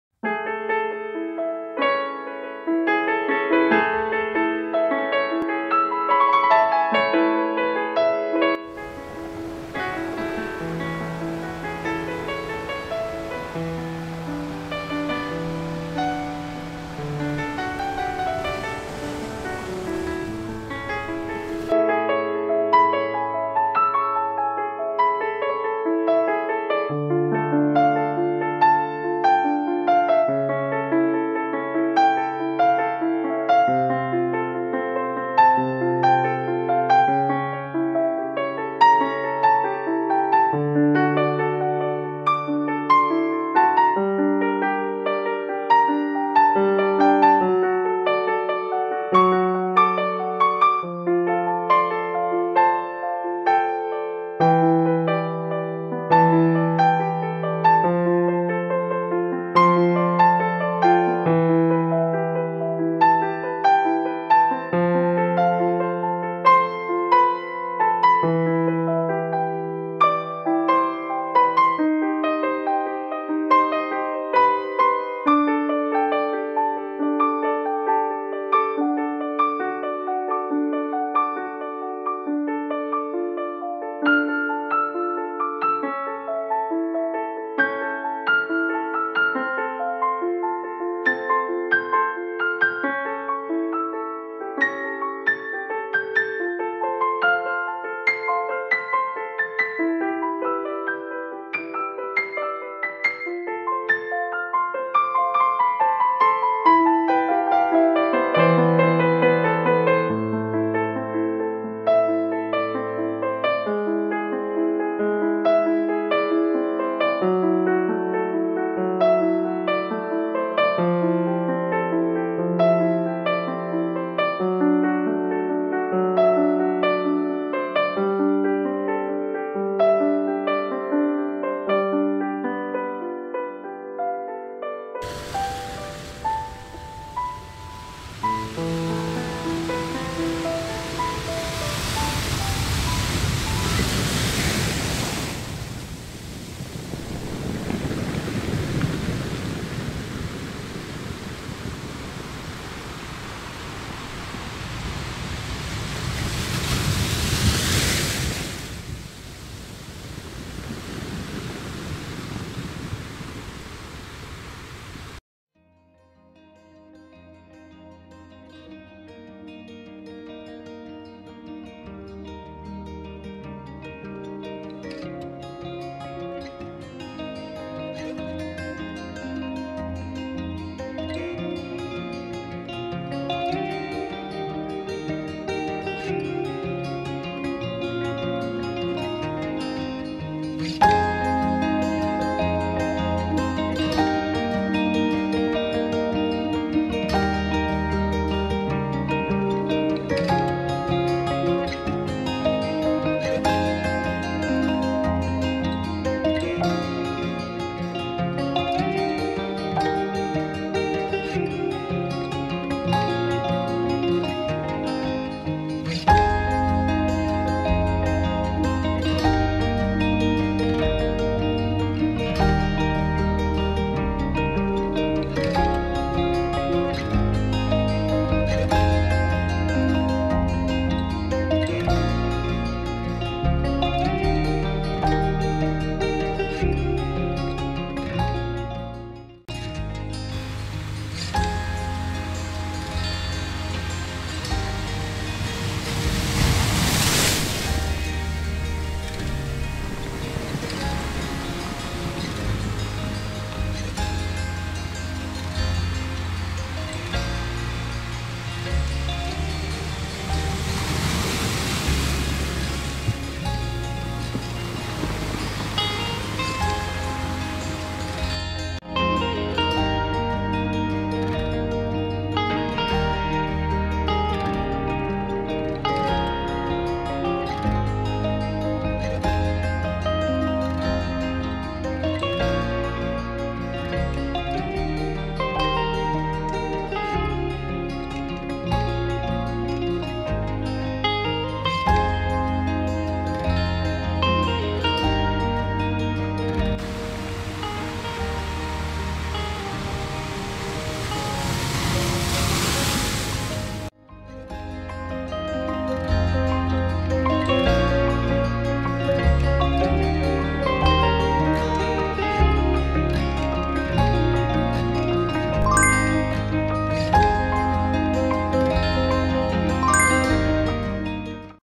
Beautiful Sunset and Inspirational Quotes
In this vlog, I am sharing my pictures of sunset with some of my favorite inspirational sunset quotes on it.